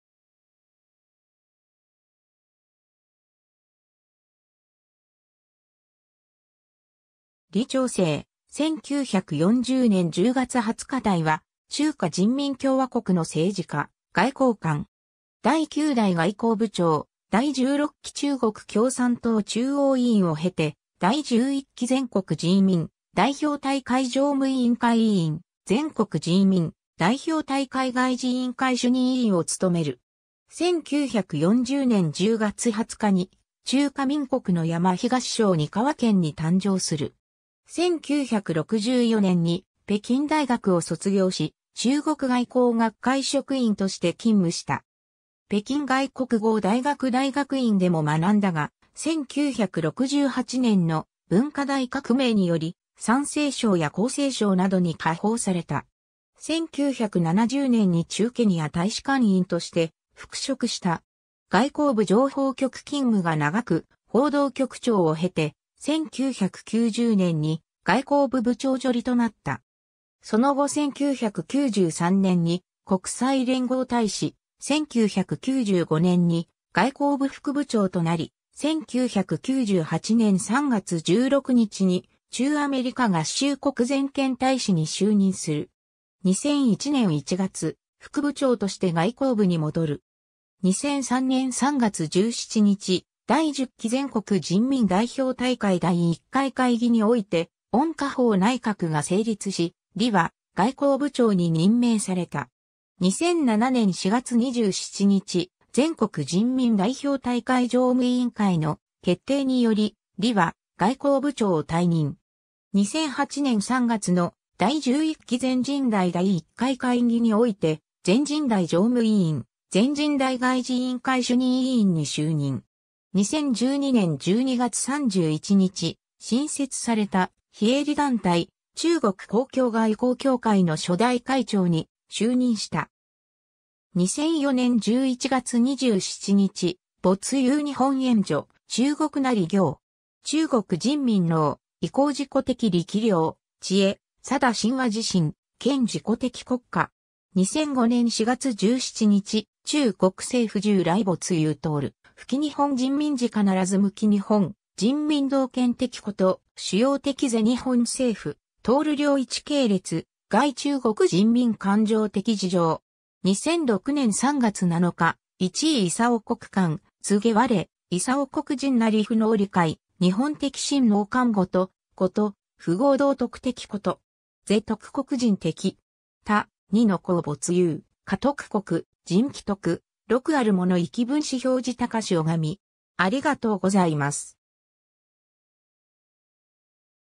李肇星
李 肇星（り ちょうせい、簡体字：李 肇星、繁体字：李 肇星、英語：Li Zhaoxing（リー・ヂャオシン）、1940年10月20日 - ）は、中華人民共和国の政治家、外交官。
第9代外交部長（外務大臣）、第16期中国共産党中央委員を経て、第11期全国人民代表大会常務委員会委員・全国人民代表大会外事委員会主任委員を務める。
 1940年10月20日に中華民国（汪兆銘政権）の山東省膠県に誕生する。
1964年に北京大学を卒業し、中国外交学会職員として勤務した。
北京外国語大学大学院でも学んだが、1968年の文化大革命により、山西省や江西省などに下放された。
 1970年に駐ケニア大使館員として復職した。
外交部情報局勤務が長く、報道局長を経て、1990年に外交部部長助理（次官補）となった。
その後1993年に国際連合大使、1995年に外交部副部長（次官）となり、1998年3月16日に駐アメリカ合衆国全権大使に就任する。
 2001年1月、副部長として外交部に戻る。
2003年3月17日、第10期全国人民代表大会第1回会議において温家宝内閣が成立し、李は外交部長（大臣）に任命された。
 2007年4月27日、全国人民代表大会常務委員会の決定により、李は外交部長を退任。
2008年3月の第11期全人代第1回会議において全人代常務委員、全人代外事委員会主任委員に就任。
 2012年12月31日、新設された非営利団体「中国公共外交協会」の初代会長に就任した。
 2004年11月27日　“没有日本援助，中国也行。”“中国人民能够依靠自己的力量、智慧、决心和自信，建设自己的国家。” 2005年4月17日　“中国政府从来没有做对不起日本人民而必须向日本人民道歉的事，主要的问题是日本政府做了一系列伤害中国人民感情的事情。” 2006年3月7日　“一位德国官员告诉我，德国人也不能理解，日本的领导怎么能干这种事，这种傻事，不合道德的事。这是德国人说的。他们说，二战之后没有哪个德国领导人还对希特勒或者纳粹分子表示崇拜。”